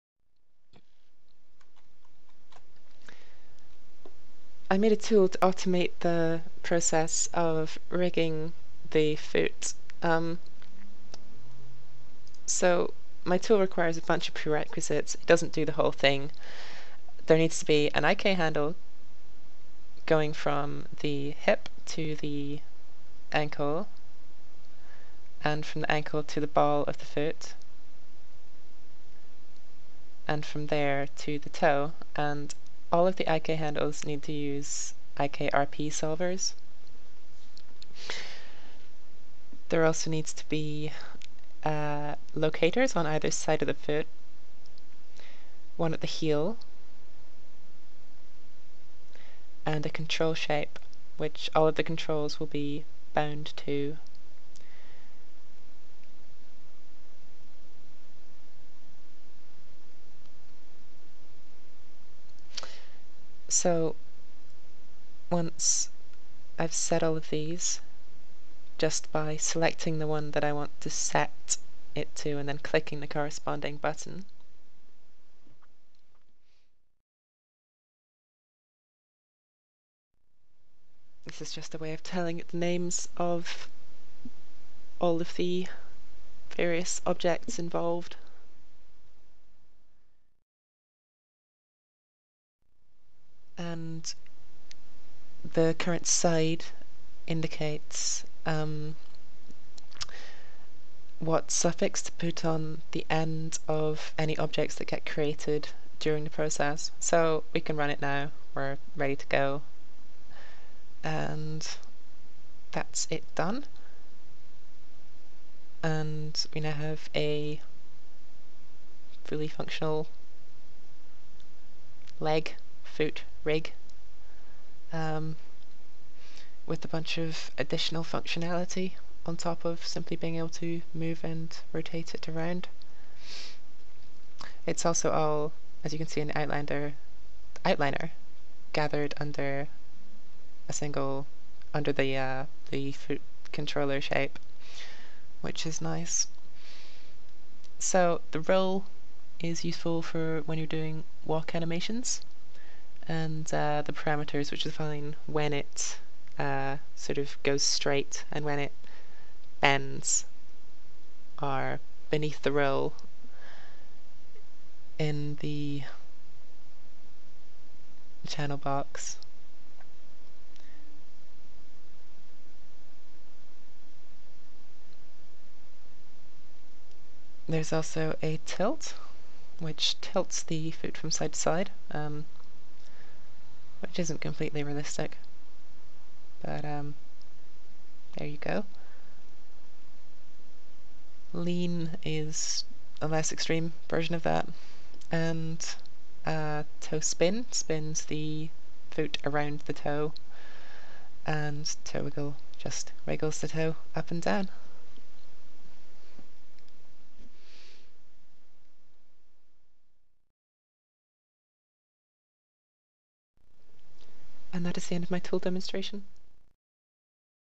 tech art tool demo
thing i made that is bad for tech art, a module on the course i'm on at university